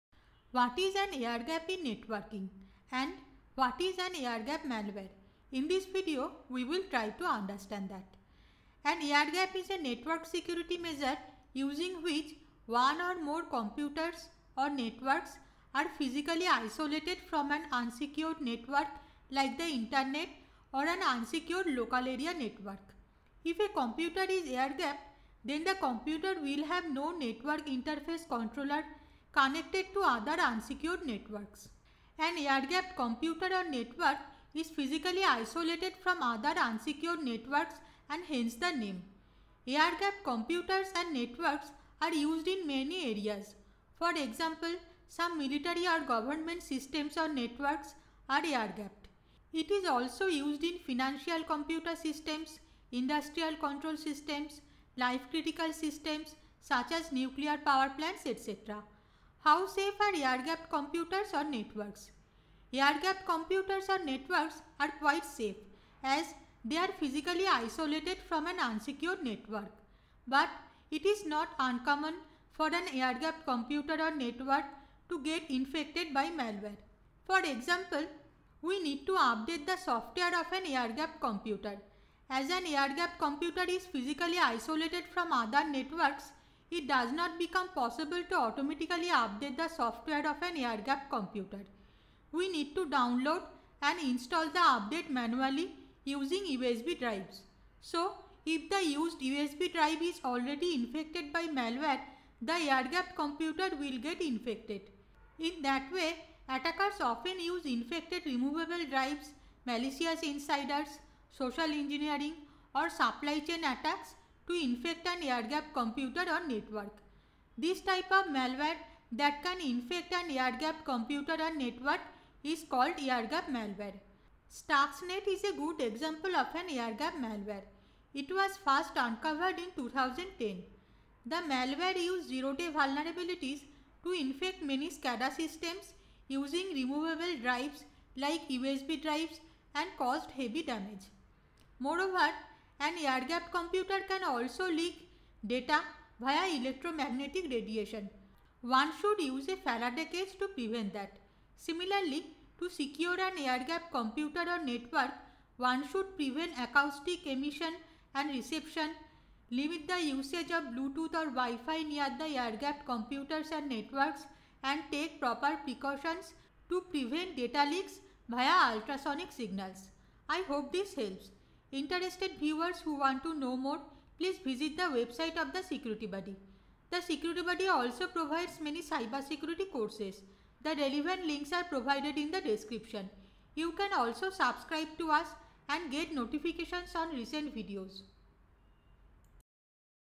What is an air gap in networking and what is air gap malware?
This video explains what an air gap in networking is and what air gap malware is. Interested viewers may find the following links useful:

What is air gap malware? - The Security Buddy

What is a Faraday Cage and how does it work? - The Security Buddy